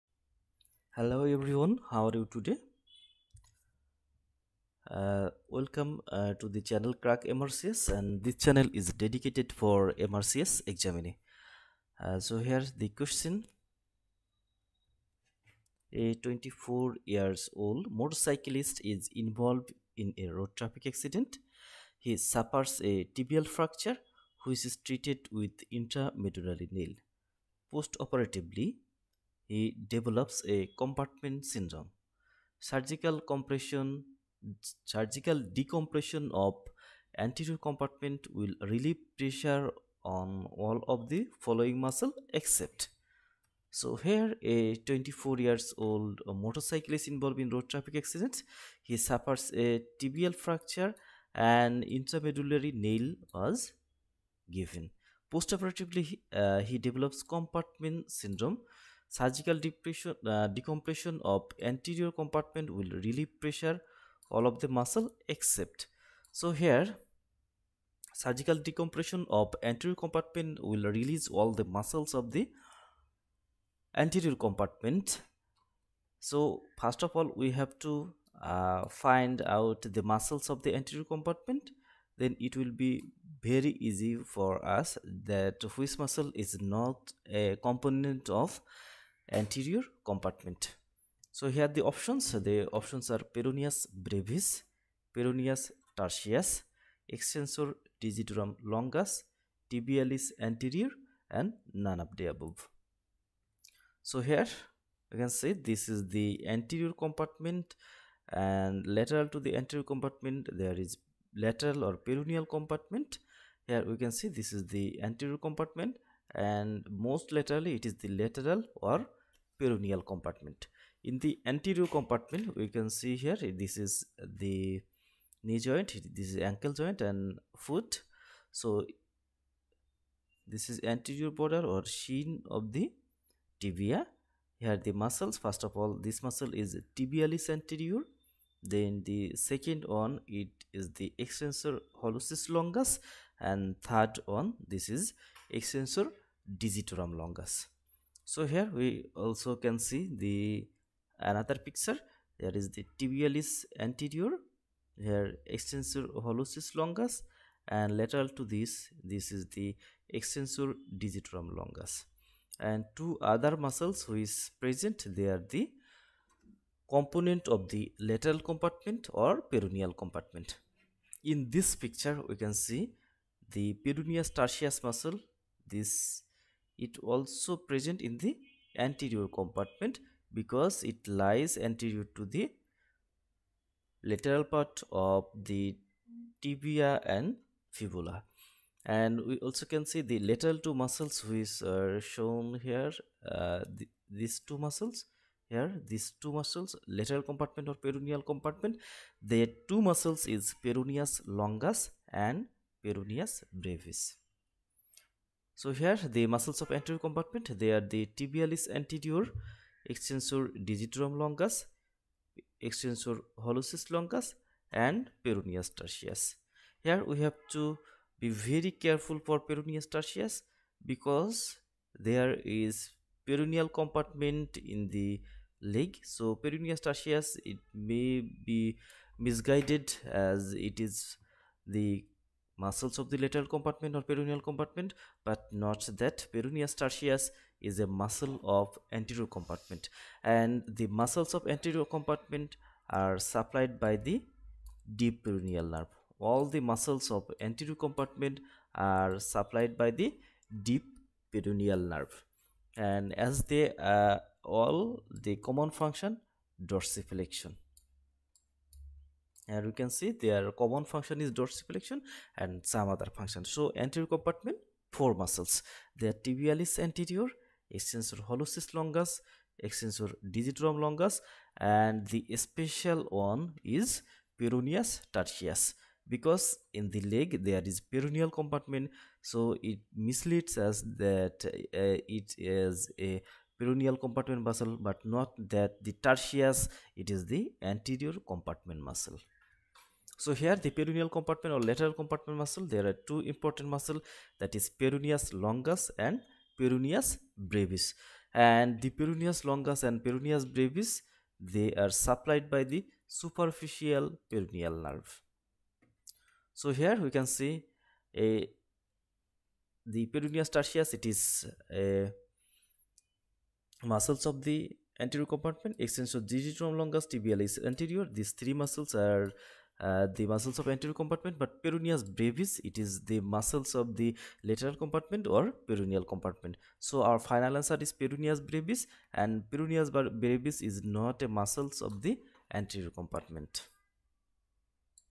Anterior compartment of leg muscle MRCS question solve
it includes
1. eMRCS videos
2. Fawzia sheet solved
3. Recalls and mores